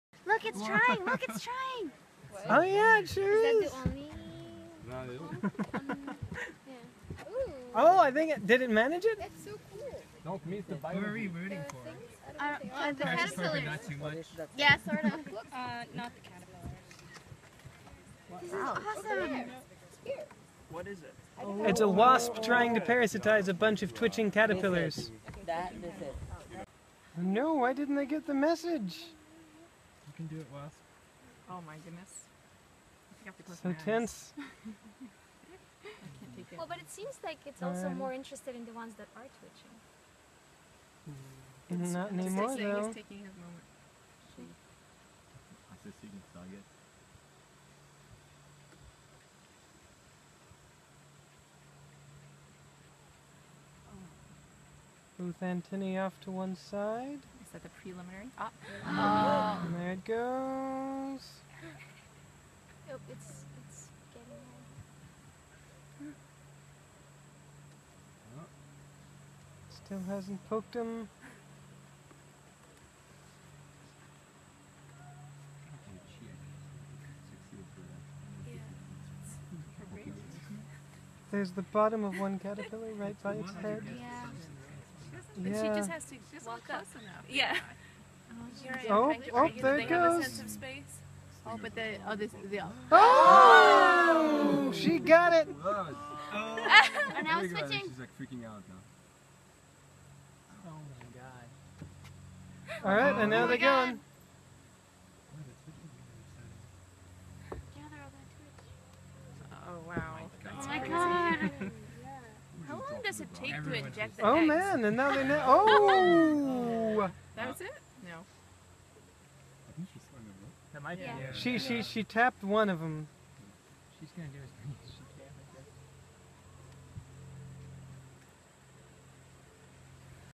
A wasp trying to parasitize a bunch of twitching caterpillars
Found whilst out walking on the Rockefeller University Social Evolution and Behaviour Course, August 2016, Millbrook NY